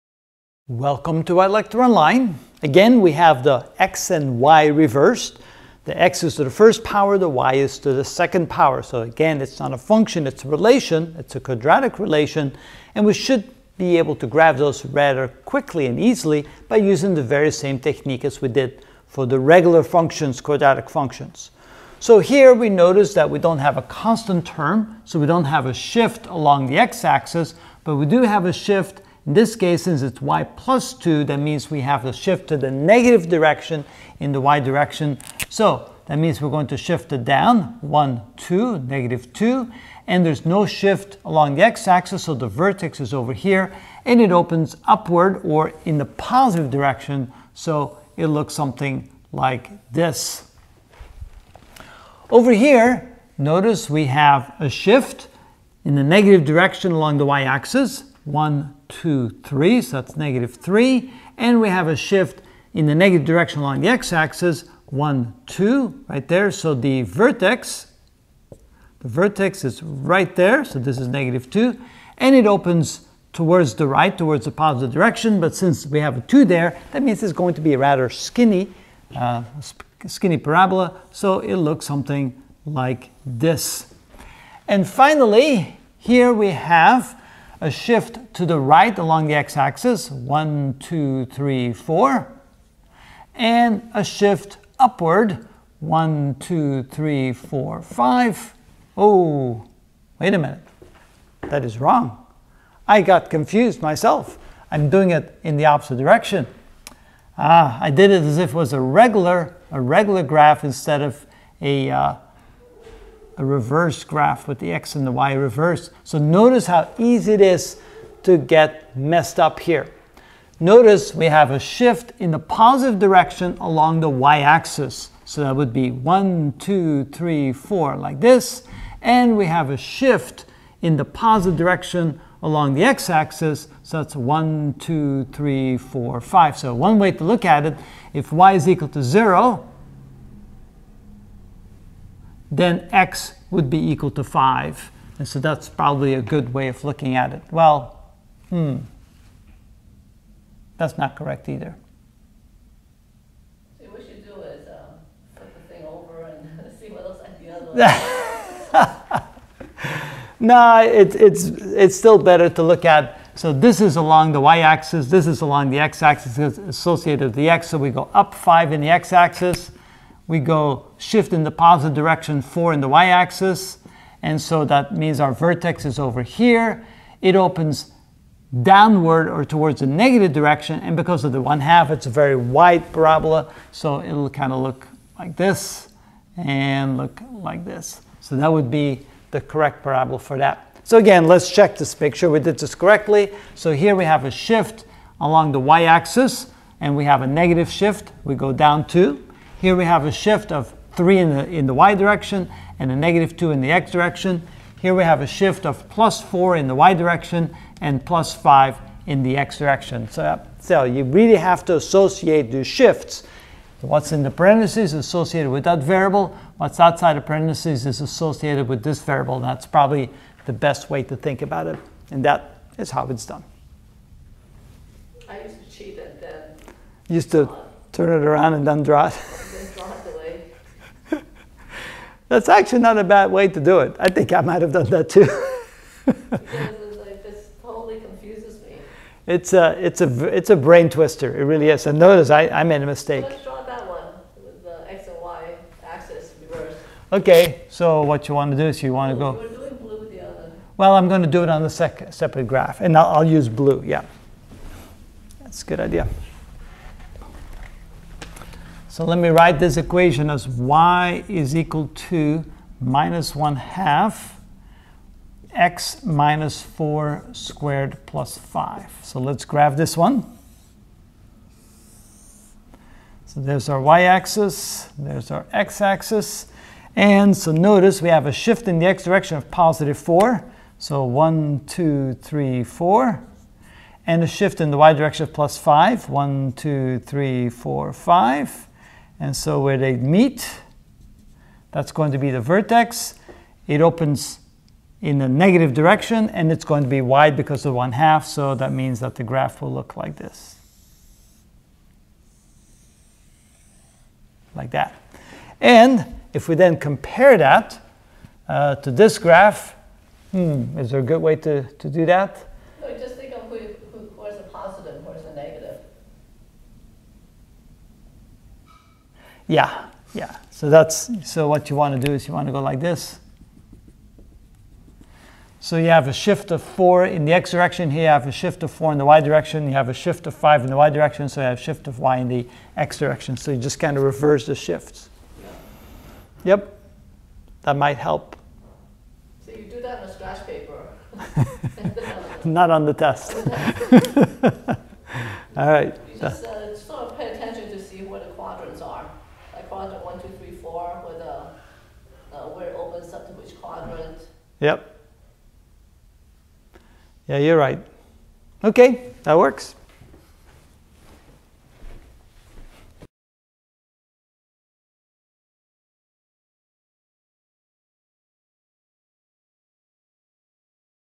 Algebra Ch 37 Parabola (17 of 22) Graph the Following Ex. 7
We will graph the following equations, in the form of y=a(x-h)^2+k, Example 7:
1. x=(y+2)^2
2. x=2(y+3)^2-2
3. x=(-1/2)(y-4)^2+5